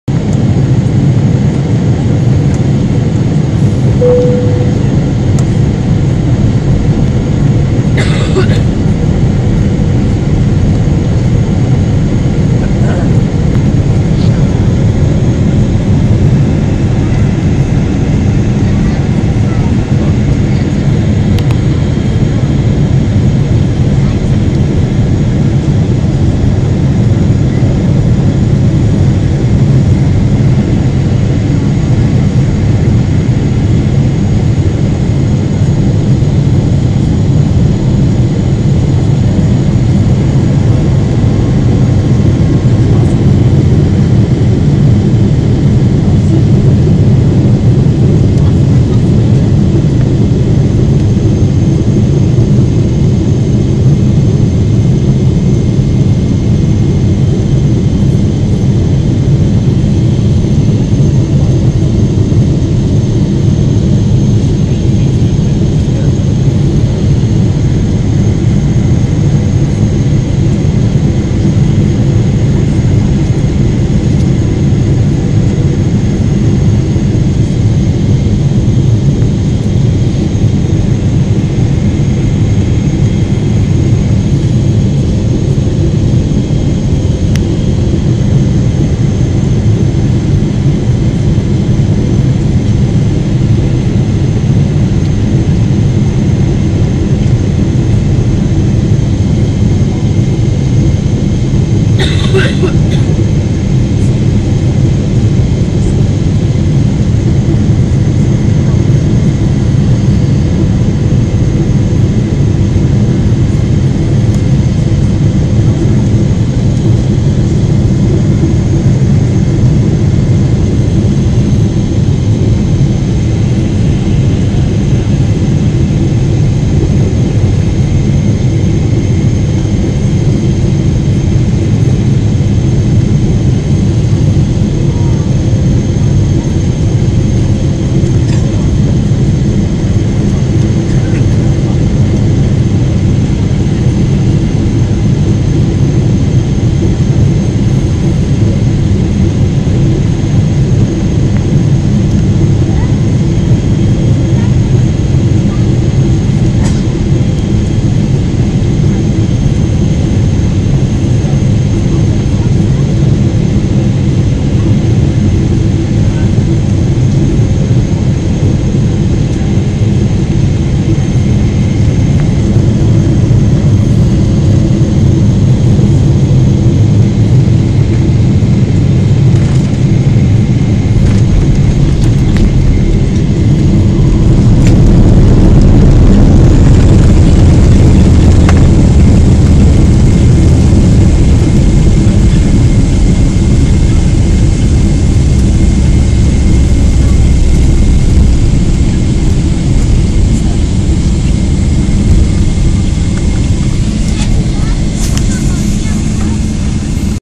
Etihad Flight ( Boeing 777-300) Abu Dhabi - Bangkok 24.07.12
approach Suvarnabhumi Bangkok Airport ...